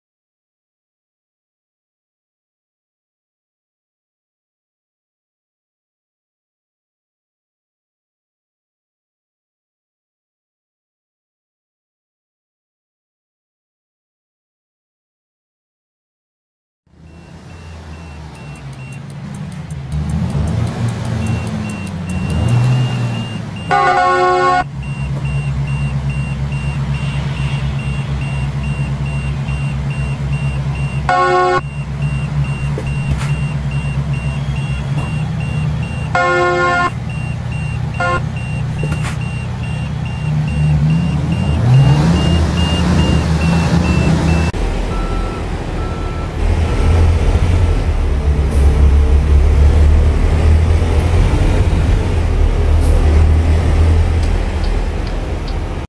1014472, nov 24
Blocking after failure to deliver trailer. My heavy trailer was a lot tougher then the default ones.